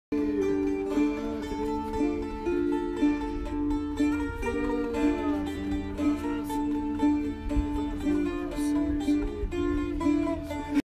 Santorini Greece, beautiful busker
October in Greece 🇬🇷

I was on a tour with a group from webjet, last stop on the tour was Santorini such spectacular views cannot fault it